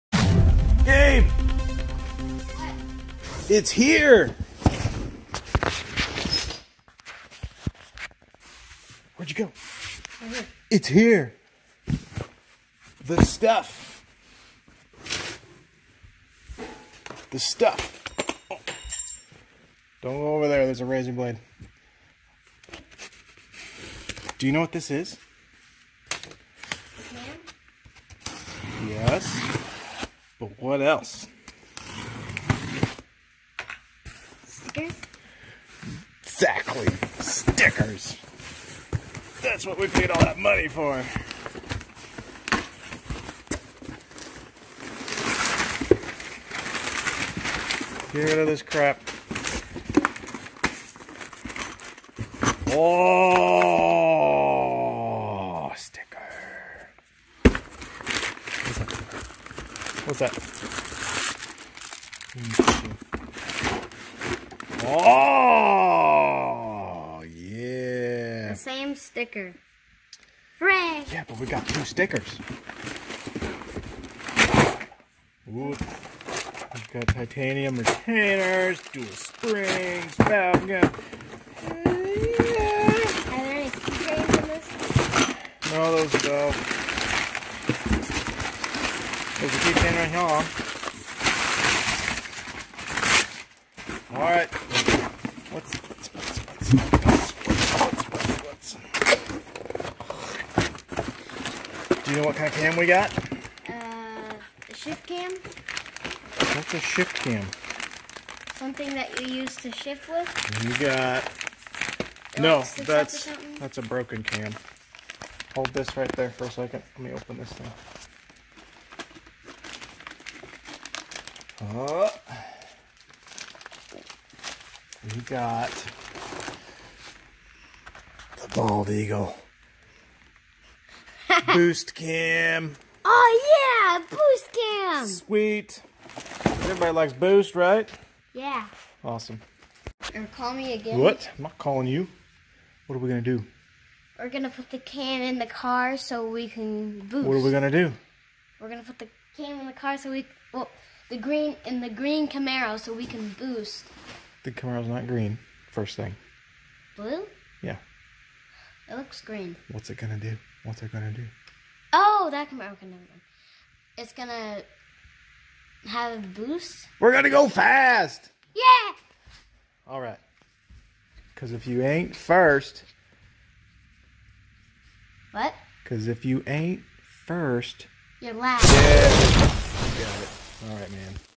TSP Bald Eagle Boost Cam has arrived for the Thirdgen Camaro 408 Stroker!
Unboxing the "stickers" (Texas Speed Bald Eagle Boost Cam) for the thirdgen Camaro boosted 408 Stroker build.
CalNaughtenJr is my hero